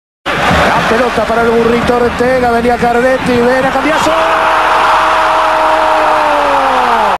Esteban Cuchu Cambiasso - River Plate vs Boca - Apertura 2001
Gol de Esteban Cuchu Cambiasso jugando para River Plate partido vs Boca Juniors en el torneo Apertura del año 2001.